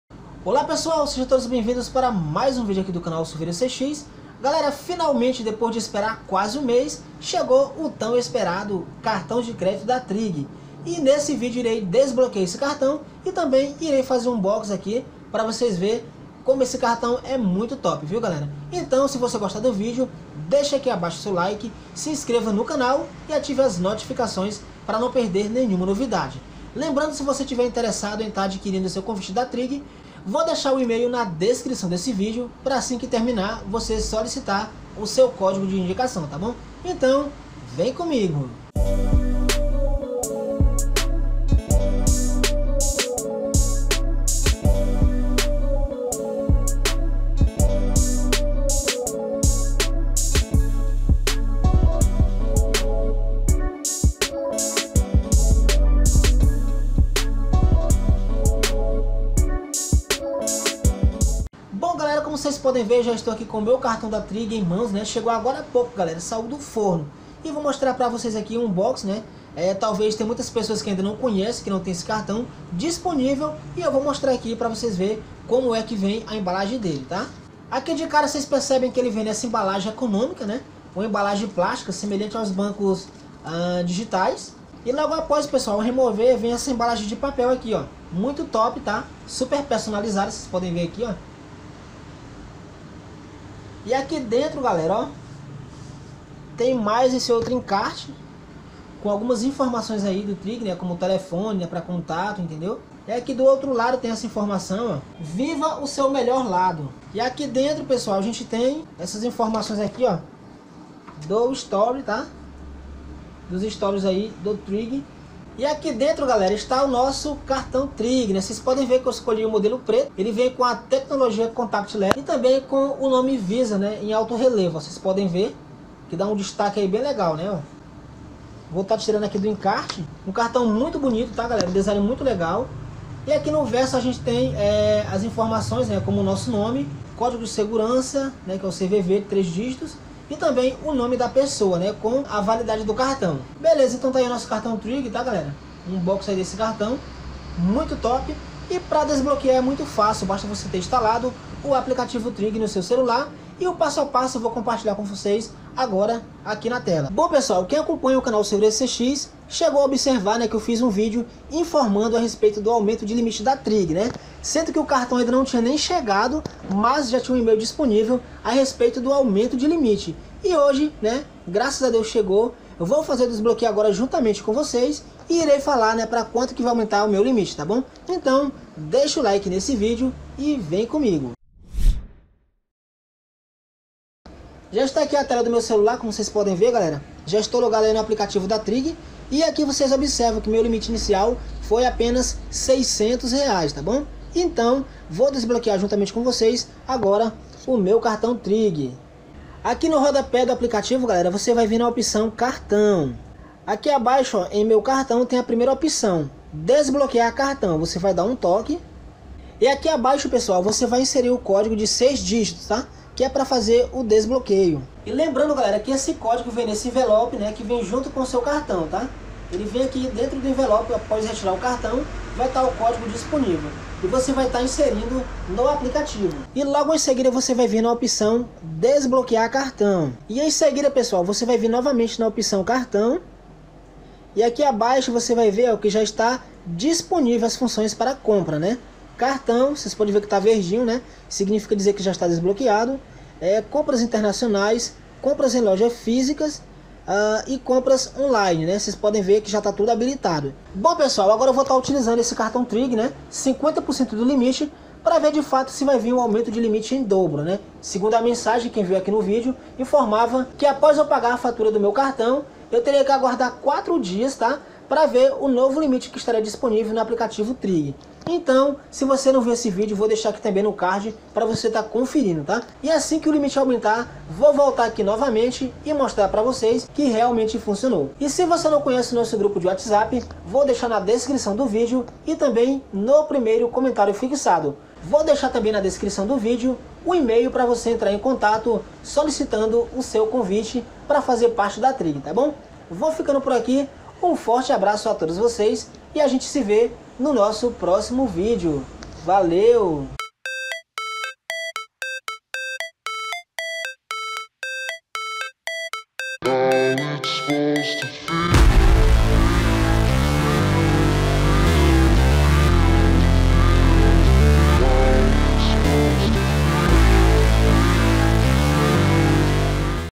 CARTÃO DE CRÉDITO TRIGG COMO FAZER DESBLOQUEIO EM 2022 + UNBOXING COMPLETO E LIMITE EM DOBRO
FINALMENTE CHEGOU MEU CARTÃO DE CRÉDITO TRIGG. NESSE VIDEO TE MOSTRO UNBOXING E TAMBÉM COMO FAZER O DESBLOQUEIO EM 2022. AINDA RECEBEI UM EMAIL DE AUMENTO DE LIMITEN EM DOBRO, SERÁ SE VEM MESMO??